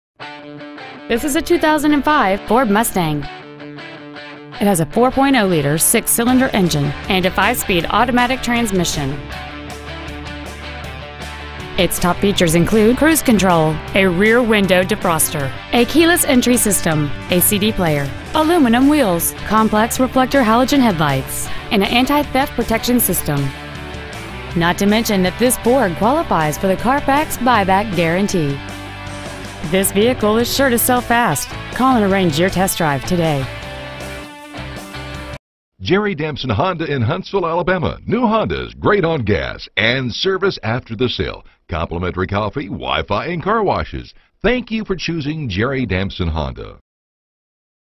2005 Ford Mustang Albertville AL
Year : 2005

 Make : Ford

 Model : Mustang

 Engine : Gas V6 4.0L/244

 Trans . : Automatic

 Exterior : Legend Lime Metallic

 Miles : 91,761

 2200 Bob Wallace Ave

 Used 2005 Ford Mustang Huntsville AL